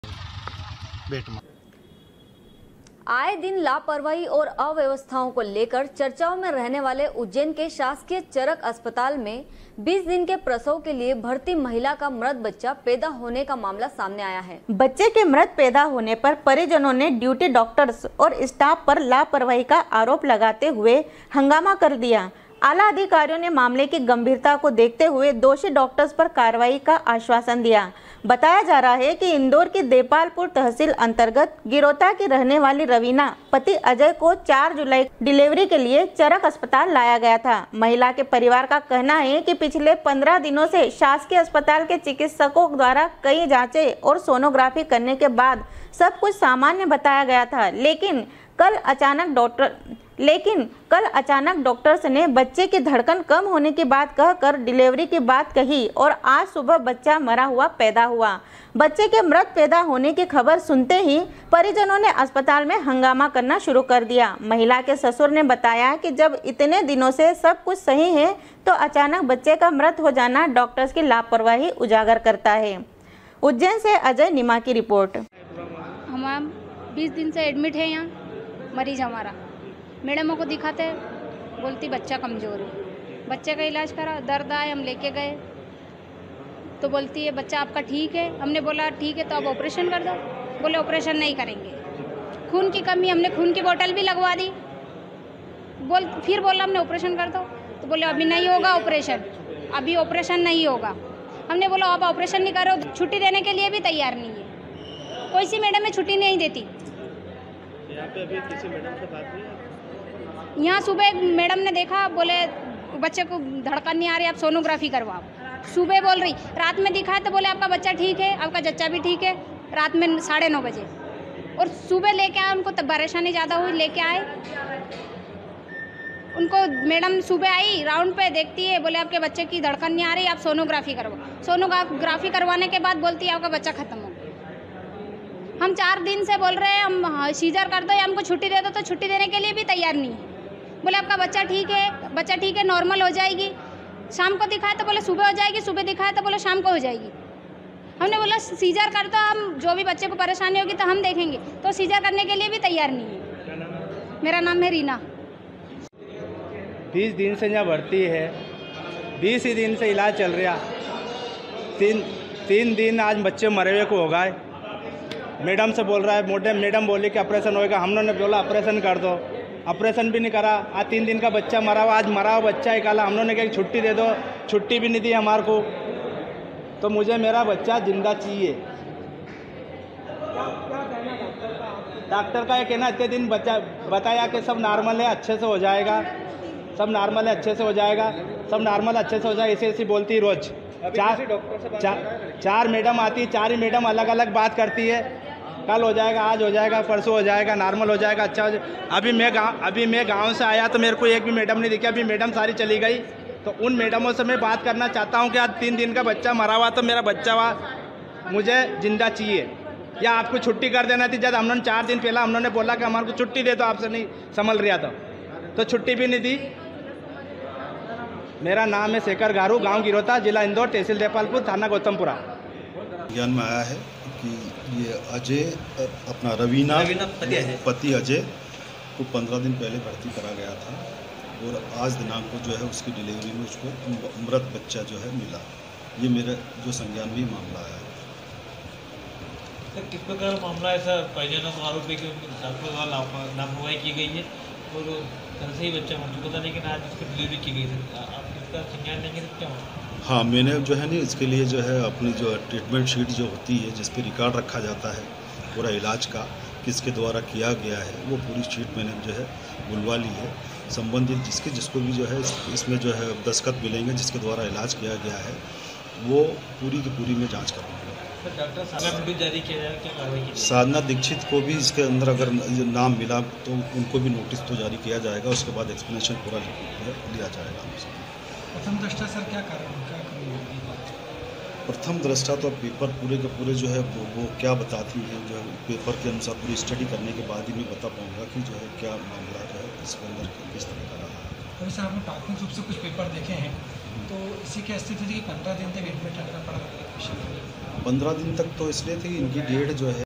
sw24news ujjain, 20.07.2019
आए दिन लापरवाही और अव्यवस्थाओं को लेकर चर्चाओं में रहने वाले उज्जैन के शासकीय चरक अस्पताल में 20 दिन से प्रसव के लिए भर्ती महिला का मृत बच्चा पैदा होने का मामला सामने आया हैsw24news